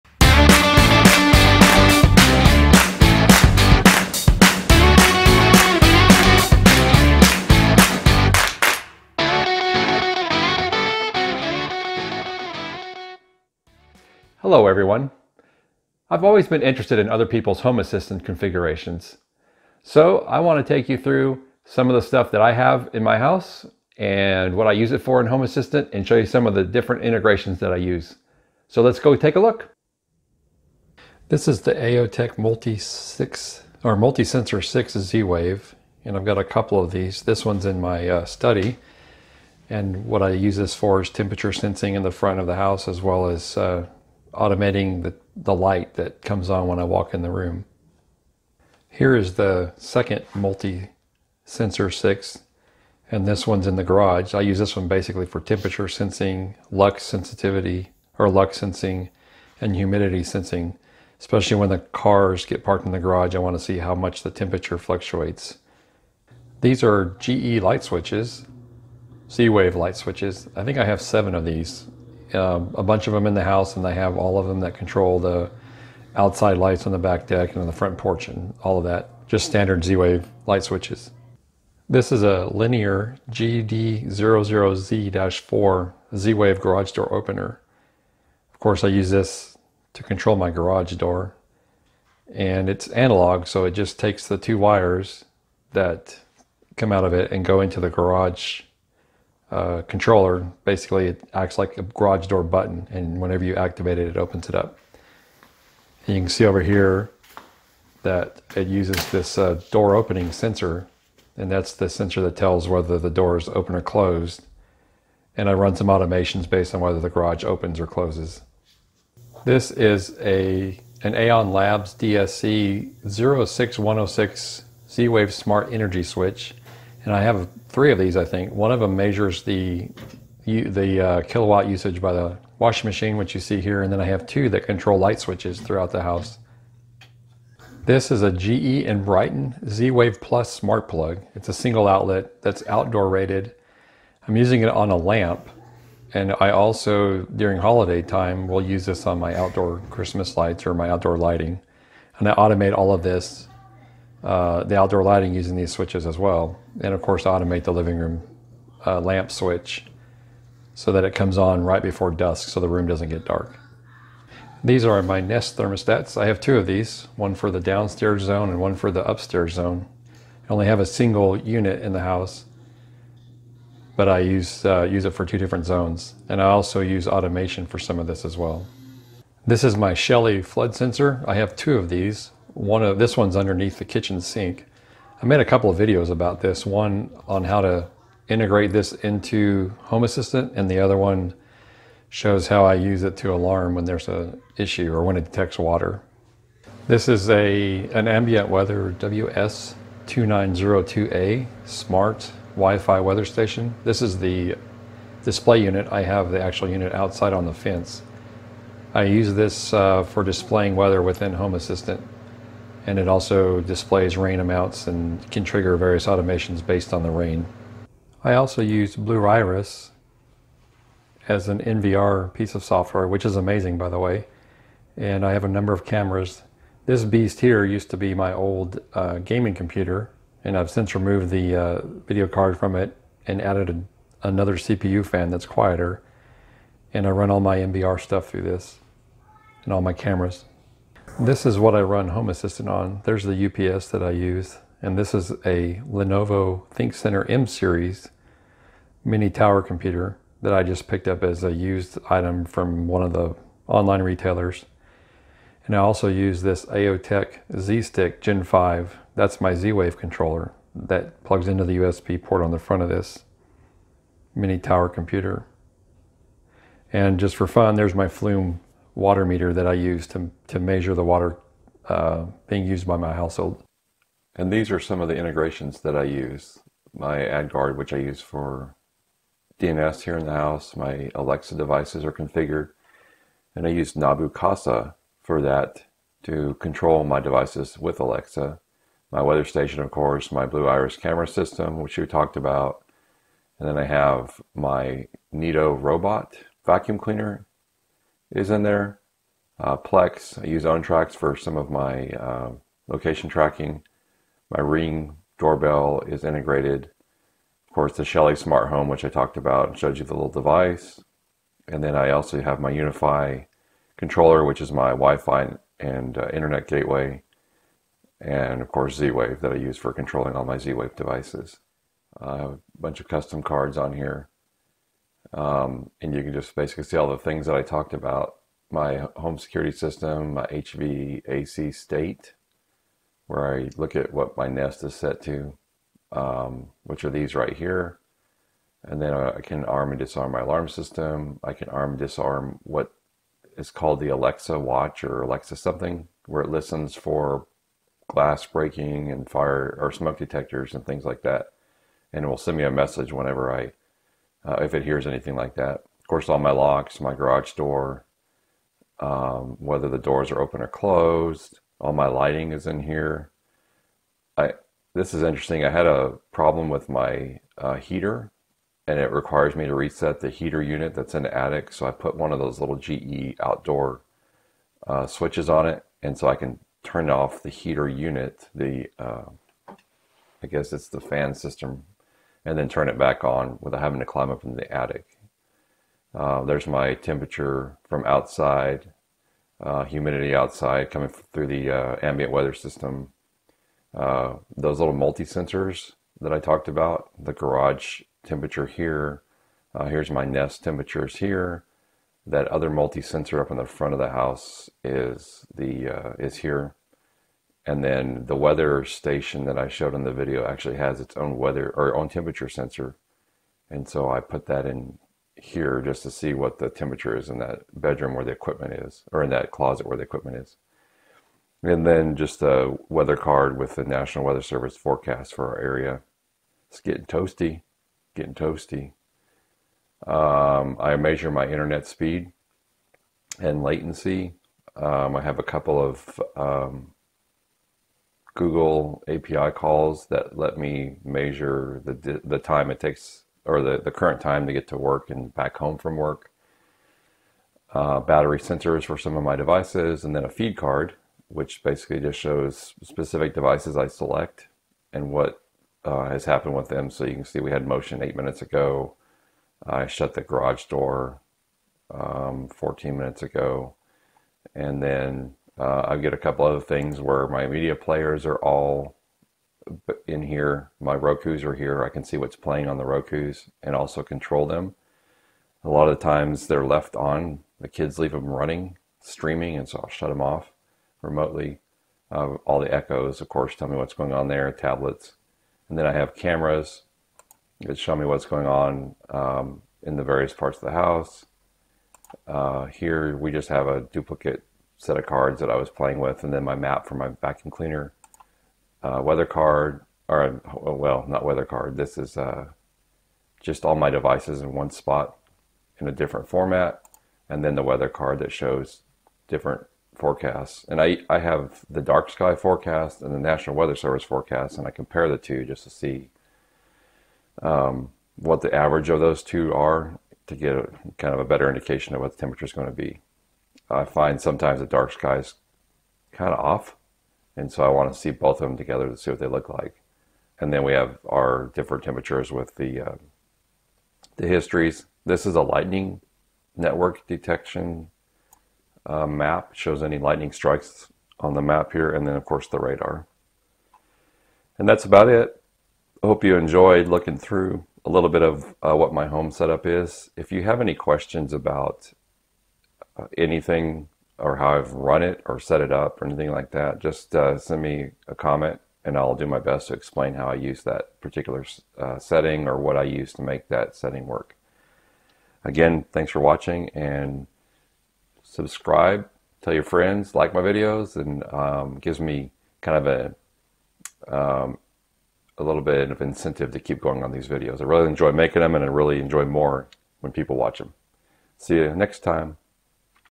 My Personal Home Assistant Setup
We take a brief look at parts of my home assistant setup. I show you some hardware I use as well as a sample of my dashboard and integrations.

Mostlychris
24165 IH-10 West